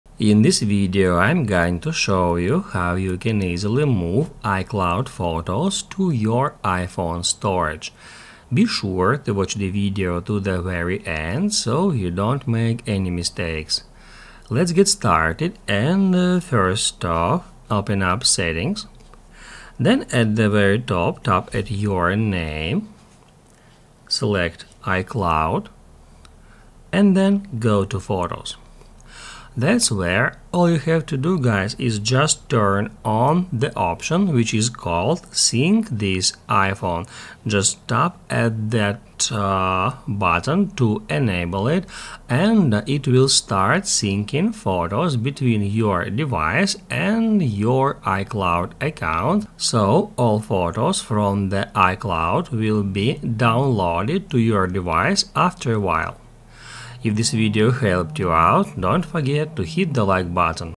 How to Move iCloud Photos to iPhone 15 Pro Storage - Full Guide
Here's how to move icloud photos to iphone 15 pro storage